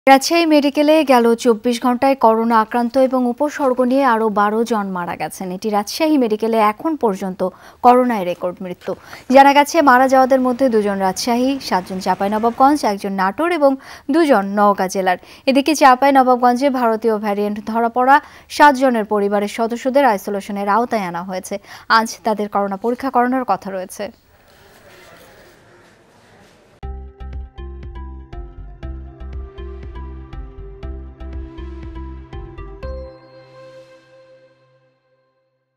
রাজশাহী মেডিকেলে করোনায় একদিনে আরও ১২ জনের মৃত্যু! | Rajshahi Corona
রাজশাহী মেডিকেলে গেল ২৪ ঘণ্টায় করোনা আক্রান্ত এবং উপসর্গ নিয়ে আরও ১২ জন মারা গেছেন।